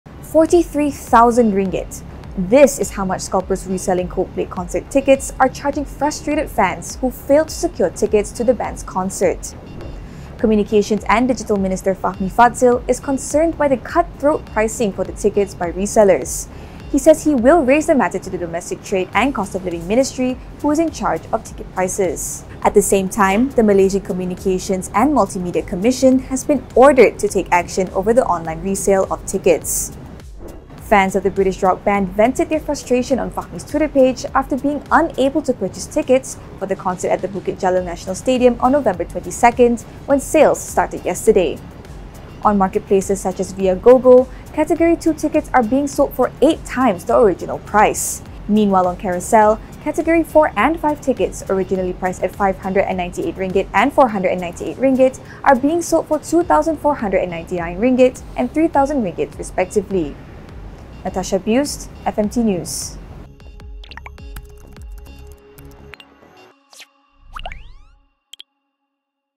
RM43,000 for Coldplay tickets just too much, says Fahmi
The communications and digital minister plans to discuss the issue of scalpers with the domestic trade and cost of living ministry.